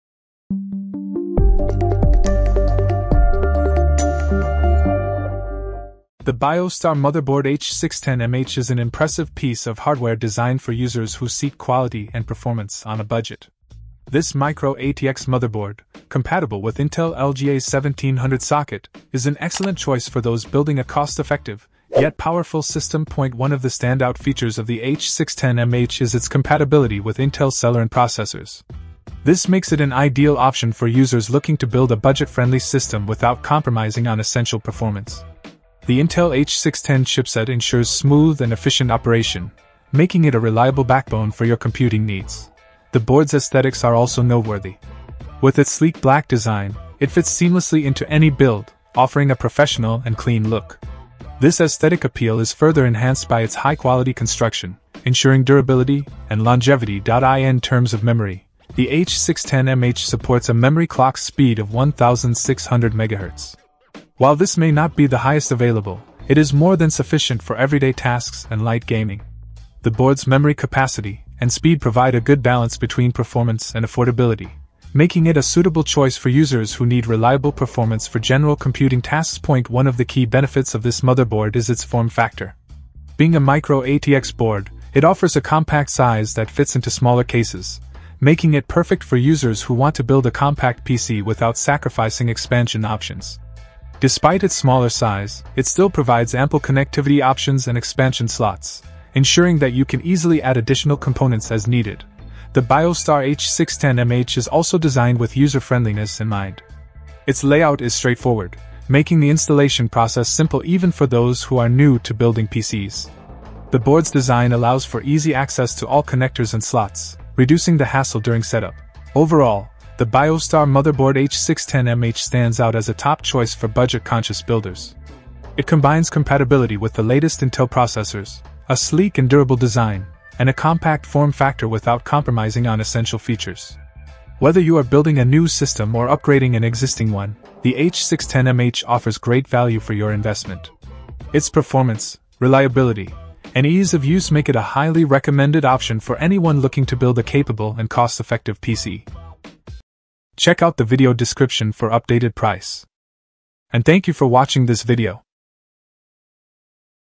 Biostar Motherboard H610MH: The Best Budget Intel LGA 1700 Micro ATX Option
0:00 Intro
0:06 Review

Things that we mentioned in this video:
Biostar Motherboard H610MH (Intel : B09W1BG779